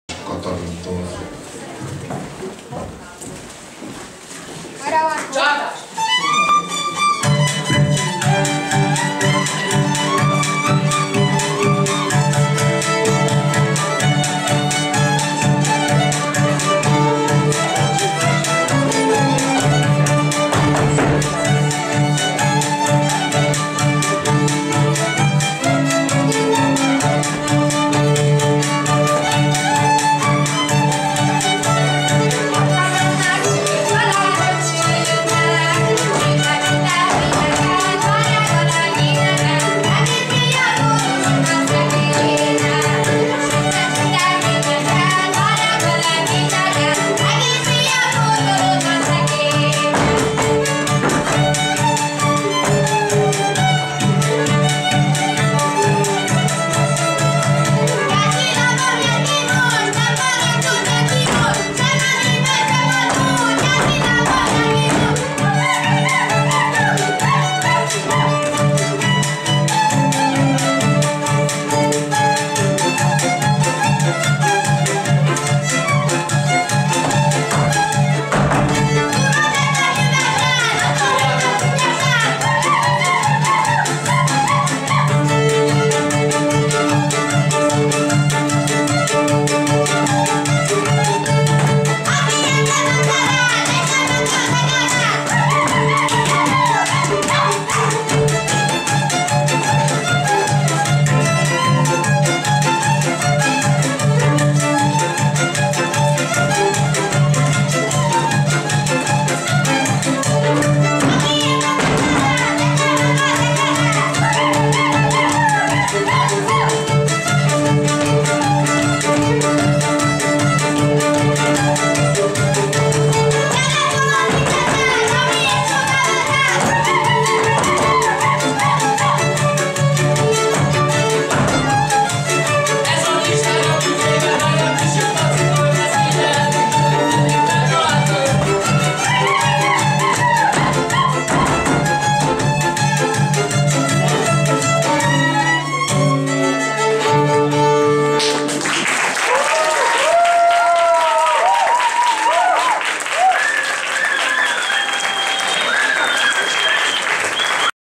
Polics Márkó - Takács Katalin: Felcsíki lassú és friss csárdás
Regionális Szólótánc Verseny, Szenna
2017. november 4.
Kísér: a Somogy zenekar